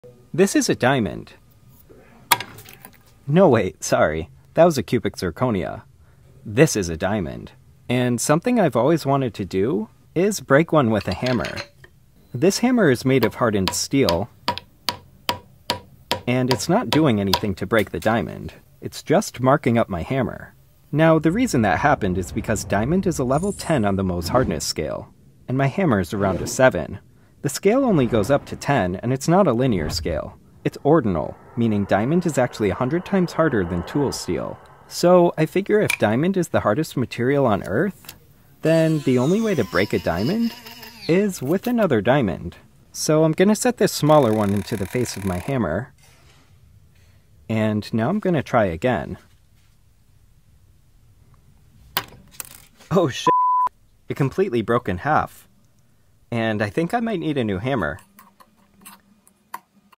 Diamond vs Hammer
I’ve always wanted to try the infamous diamond vs hammer experiment… so I did!